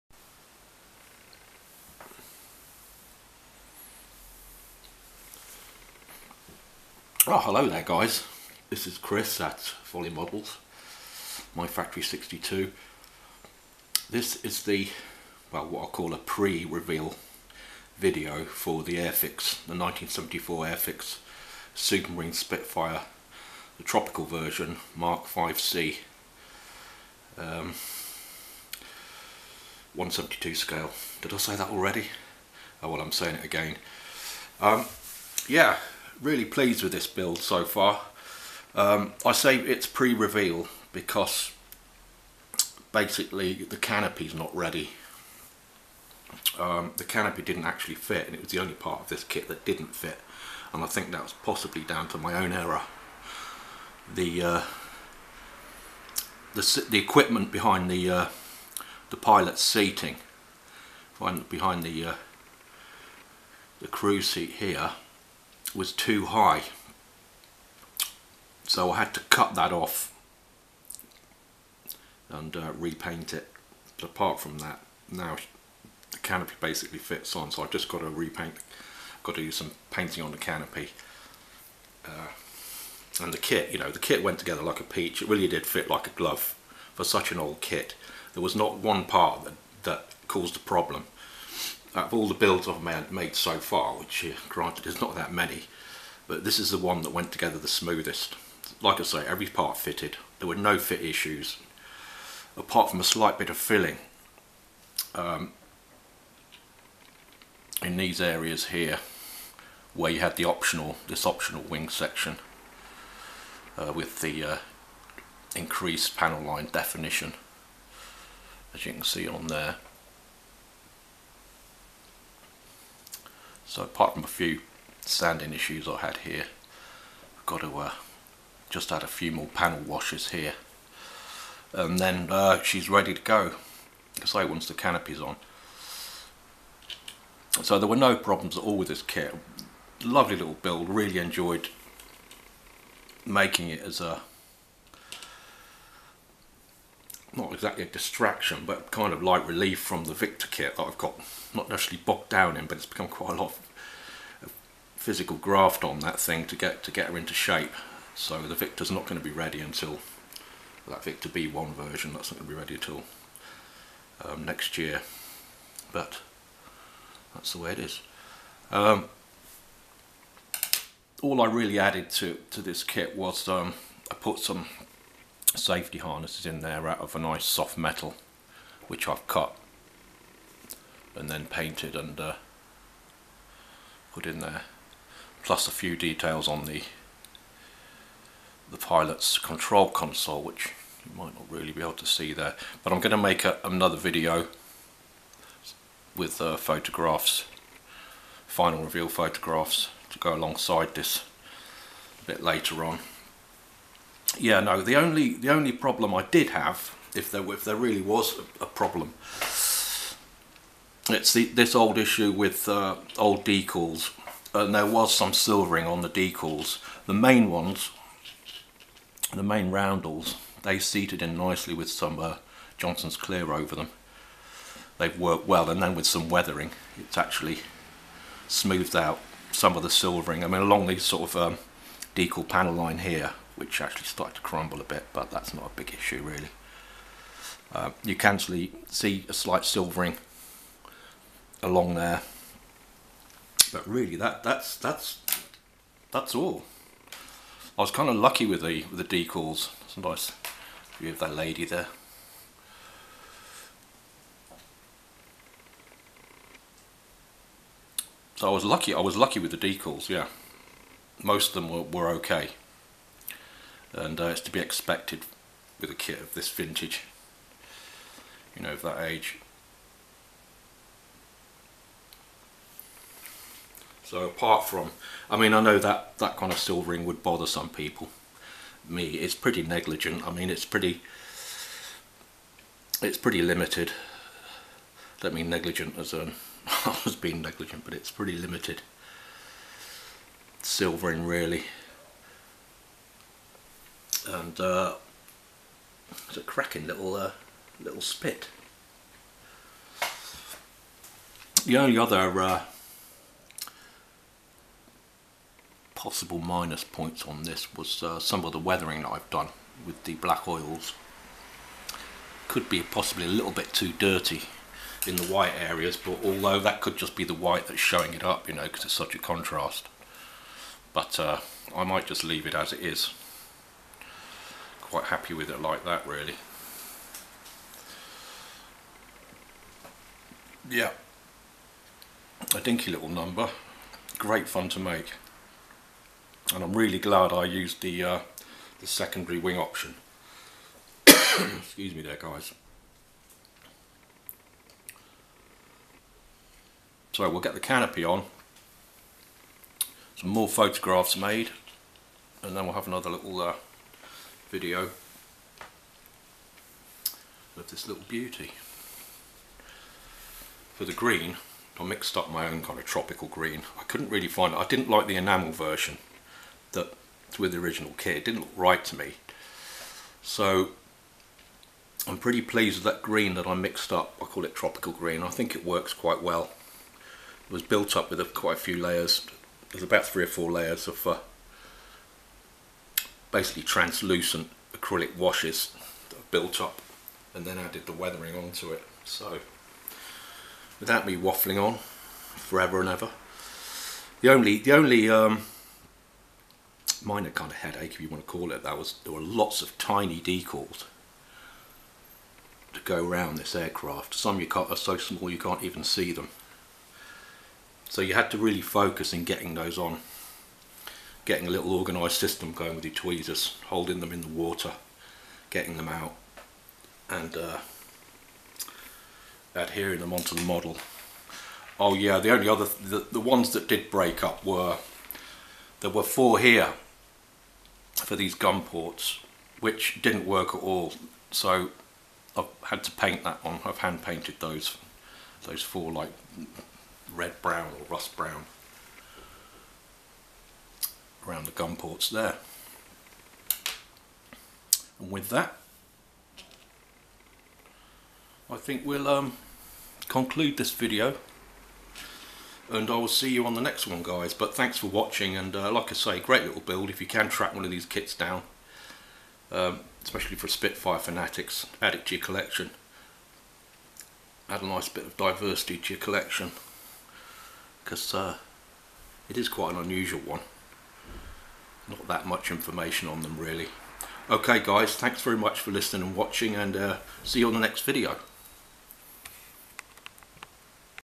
MFM: 1974 1/72 Airfix SUPERMARINE SPITFIRE MK Vc Pre-Reveal
RAAF Spitfire: tropical.1944,79 Sqdn, Merauke,New Guinea.